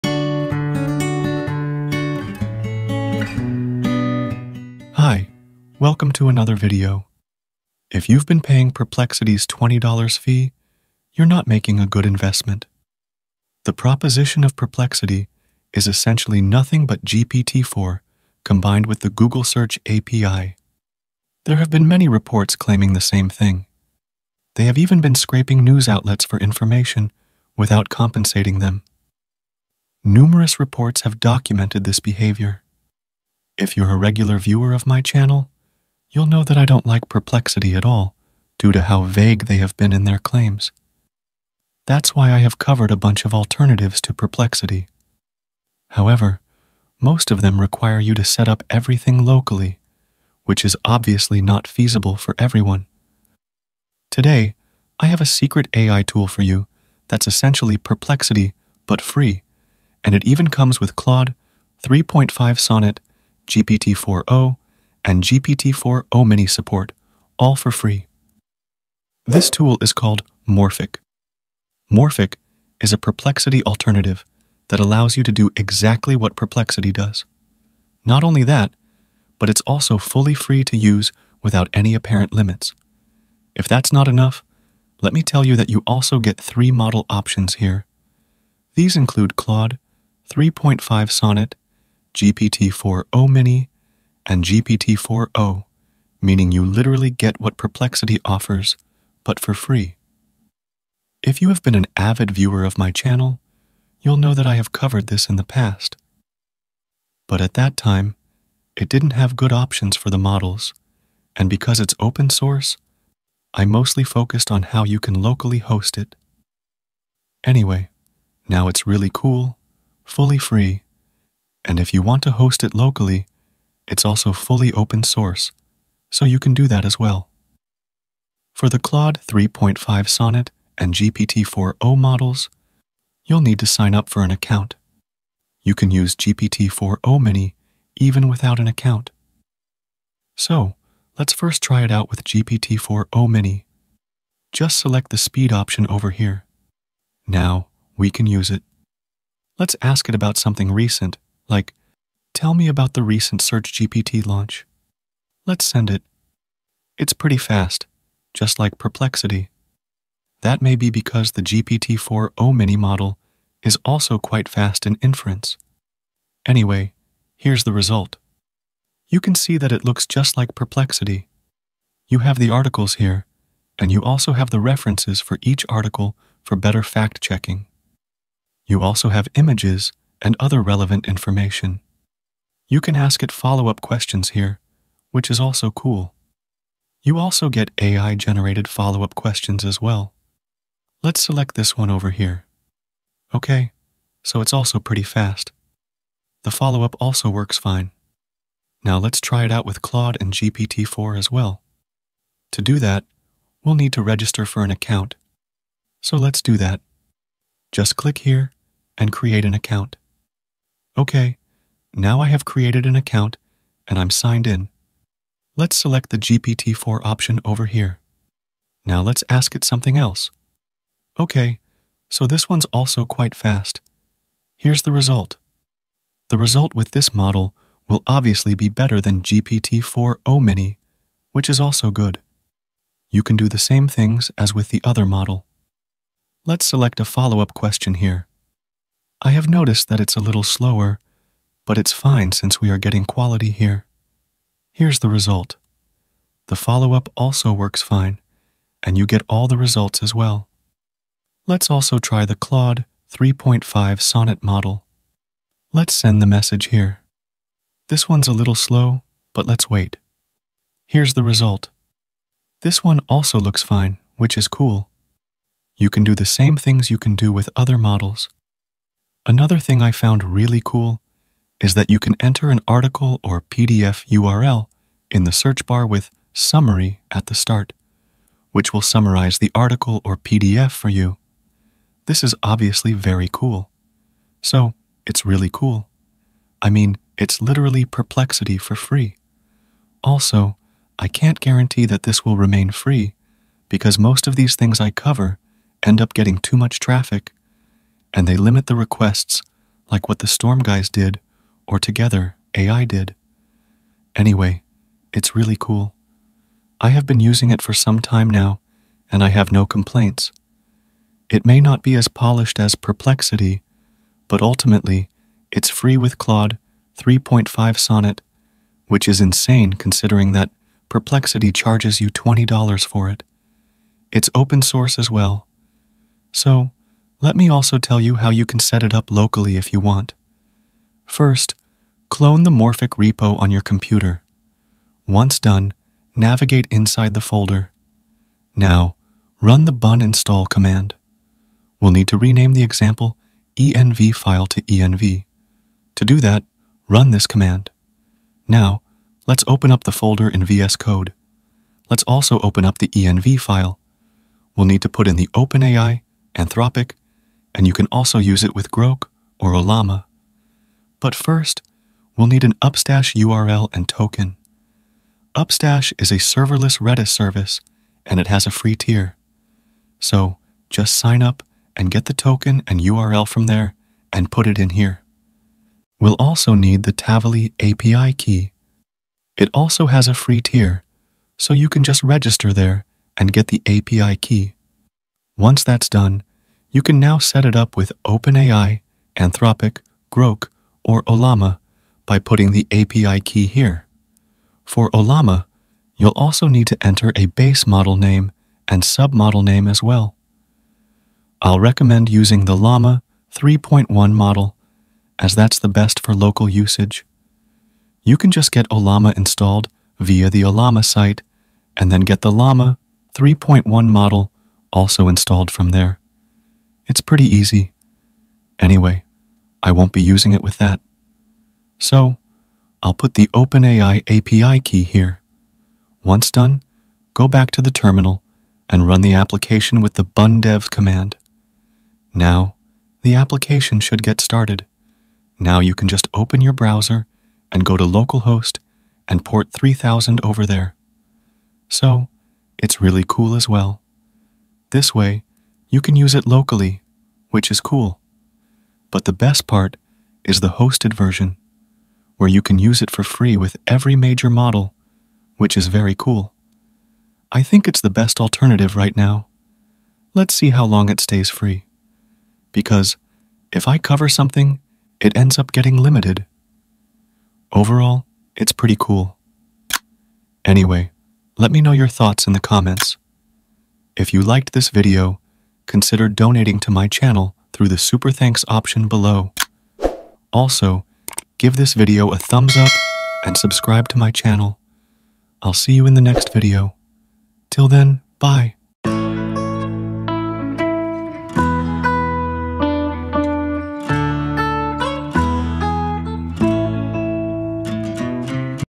This 100% FREE Perplexity Clone has GPT-4O & Claude-3.5-Sonnet! (No Installation Needed)
In this video, I'll be talking about a new, secret & free AI tool that is a perplexity clone with Claude-3.5-Sonnet, GPT-4O & GPT-4O-Mini. It gives you access to all the models for free without paying anything. This is the best FREE Perplexity Alternative which is accessible for fully free. You can stop paying for Perplexity with this secret AI tool as well. This tool is called as Morphic. Morphic is a fully free perplexity alternative with 100% FREE access to Claude-3.5-Sonnet, GPT-4O & GPT-4O-Mini.It does this all via agents / experts. It's fully free and is a kind of Perplexity Pages alternative but it is insanely better. It uses GPT-4O & Claude 3.5 Sonnet to do this. You can also use it Locally if you want as it's an Opensource Project.

Resources:
Morphic : morphic.sh
Old Video (Detailed Installation) :

Key Takeaways:

🌟 Discover Morphic: The ultimate Perplexity alternative that combines GPT-4, Claude-3.5 Sonnet, and GPT-4O models—all for free!

🔍 No More Fees: Stop paying $20 for Perplexity when Morphic offers the same features, including the Google Search API, at no cost.

⚡ Fast and Efficient: Experience the speed and accuracy of Morphic's GPT-4O-Mini model, perfect for quick searches and reliable results.

📚 Advanced Models: Get access to advanced AI models like Claude-3.5 Sonnet and GPT-4O for high-quality responses and enhanced functionality.

🌐 Open-Source Flexibility: Easily set up Morphic locally with its open-source framework, offering customizable and versatile usage options.

🔧 User-Friendly Setup: Follow a straightforward guide to install and configure Morphic on your computer, including integrating Upstash and Tavily for seamless performance.

🆓 Totally Free: Enjoy unlimited use of Morphic's AI capabilities without any apparent limits, making it the best free Perplexity alternative available today.

Timestamps:

00:00 - Introduction
01:09 - About Morphic & How to Use
05:23 - Local Installation (Optional)
07:49 - Ending